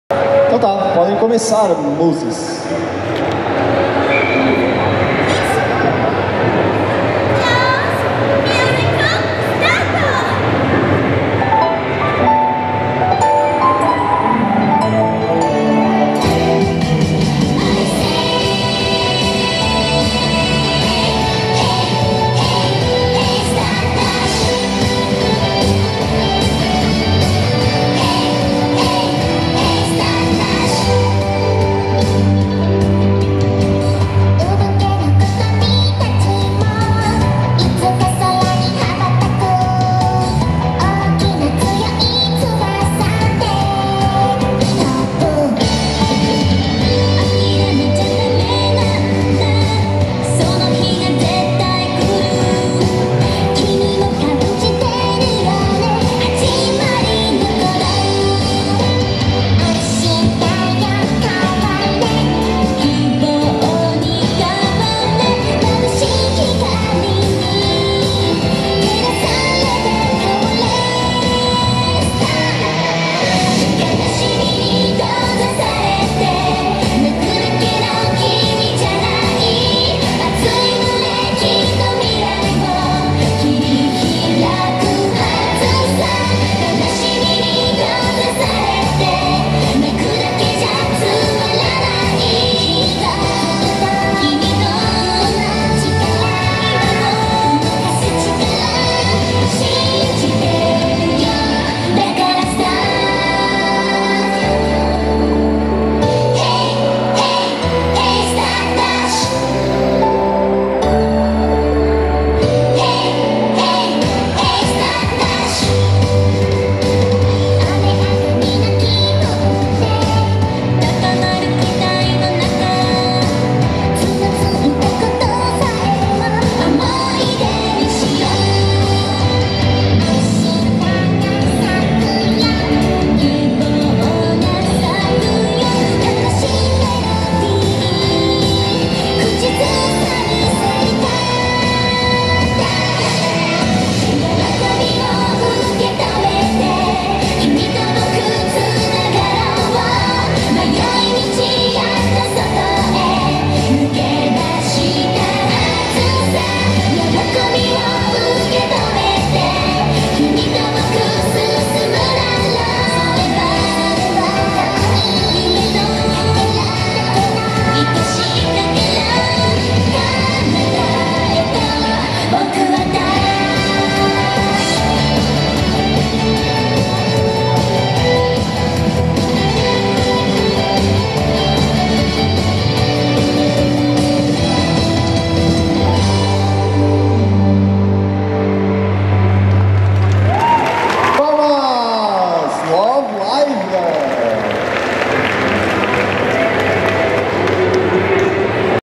Anime Gakuen 2015 - Concurso de cosplay em grupo: Love Live
Grupo de Love Live, 2º lugar no concurso de cosplay do Anime Gakuen 2015.